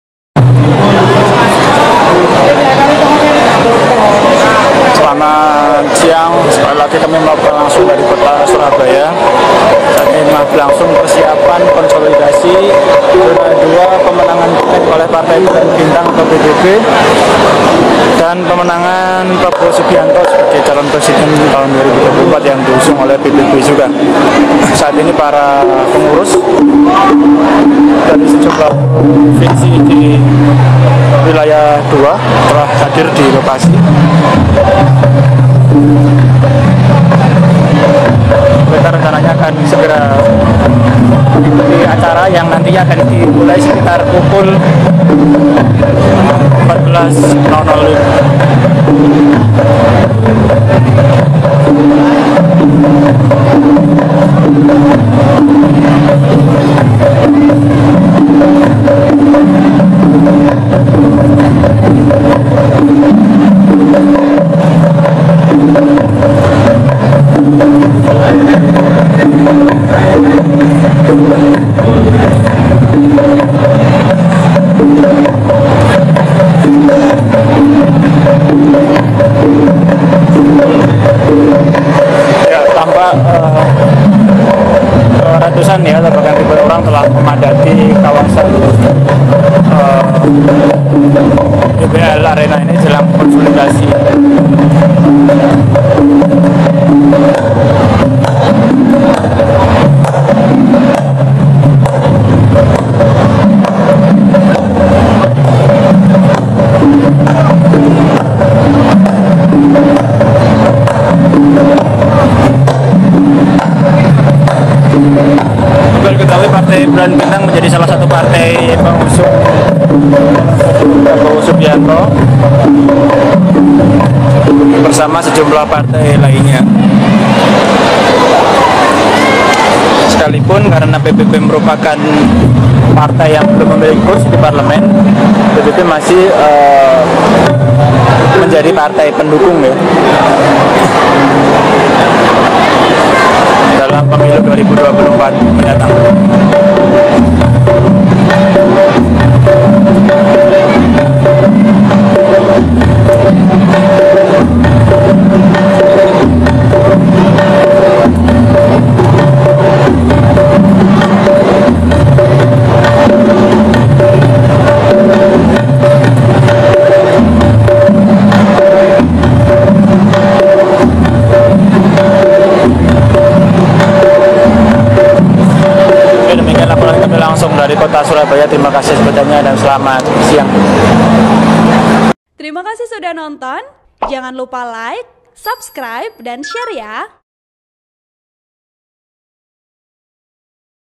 PBB Gelar Konsolidasi Akbar di Surabaya, Bahas Pileg dan Pemenangan Prabowo Subianto di Pilpres 2024
VP: Agus        Rep: Yusron Naufal Putra

TRIBUNJATIM.COM, SURABAYA - Ribuan kader Partai Bulan Bintang dari berbagai wilayah di Indonesia berkumpul di Surabaya, Minggu (3/9/2023) siang. Kegiatan konsolidasi akbar untuk persiapan Pileg ini, juga digelar untuk pemenangan Prabowo Subianto di Pilpres 2024.

Agenda ini merupakan Konsolidasi Zona II Pemenangan Pileg PBB dan Pilpres yang diikuti peserta dari 12 Dewan Pimpinan Wilayah (DPW) berikut pengurus DPC. Diantaranya, meliputi Jawa Timur, Jawa Tengah, Jawa Barat. Kawasan Bali, Kalimantan, hingga NTT dan NTB.

Dijadwalkan Ketua Umum PBB Yusril Ihza Mahendra dan jajaran DPP akan hadir secara langsung. Selain diikuti kader PBB, kegiatan ini juga dihadiri oleh perwakilan pengurus Partai Gerindra.

Dari penjelasan PBB Jatim sebelumnya, Pranowo Subianto direncanakan bakal hadir secara langsung. PBB memang menjadi salah satu partai pendukung Prabowo di Pilpres 2024.

Teriakan Prabowo Presiden pun menggema dalam kegiatan yang berlangsung di DBL Arena Kota Surabaya.